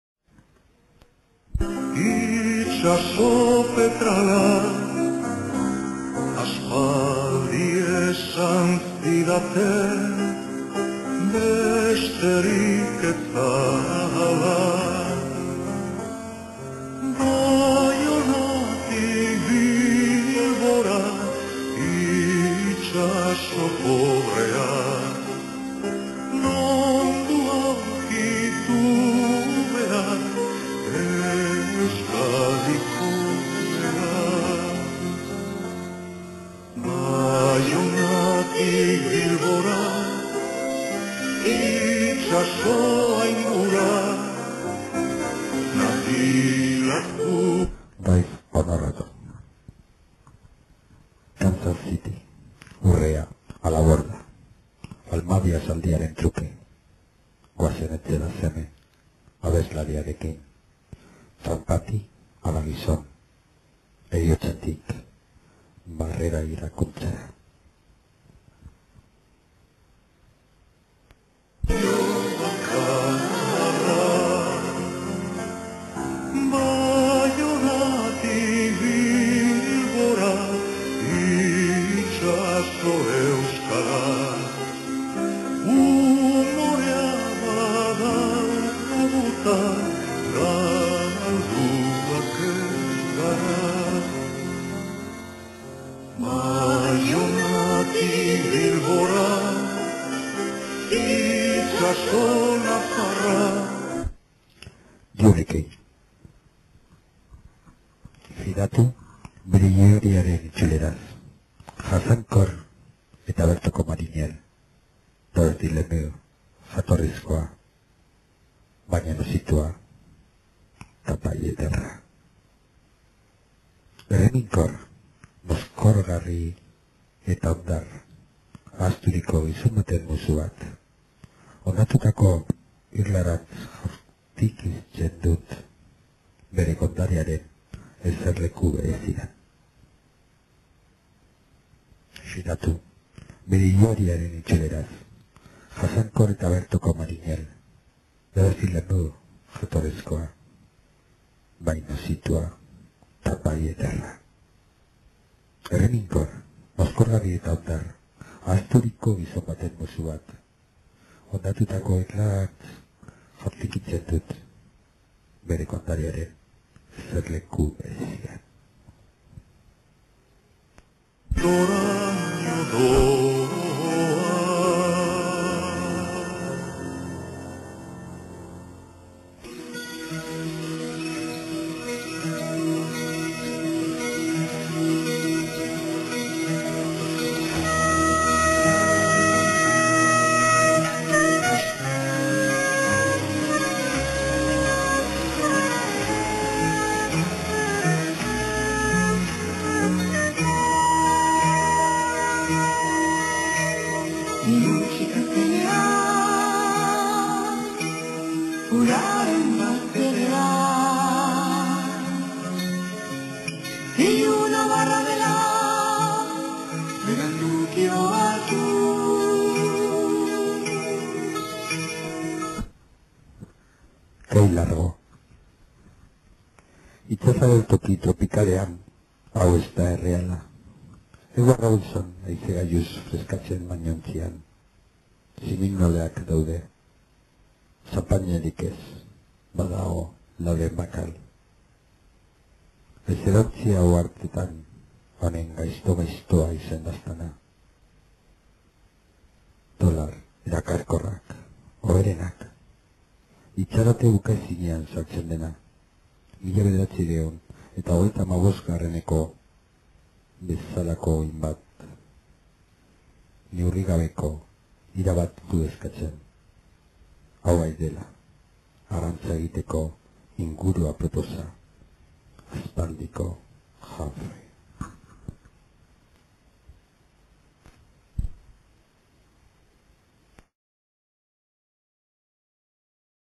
"Euskera" 4
José Ángel Pérez Berasategui, con música del País Vasco, recita en euskera (vascuence), tres poemas de Rolando Revagliatti, traducidos por Pérez Berasategui y por Iñigo Ruiz de Sabando, en Vitoria-Gasteiz, Álava, España, en 1996. Video diseñado y realizado, con imágenes relacionadas con el País Vasco, por Flavia Revagliatti, en la ciudad de Buenos Aires, la Argentina.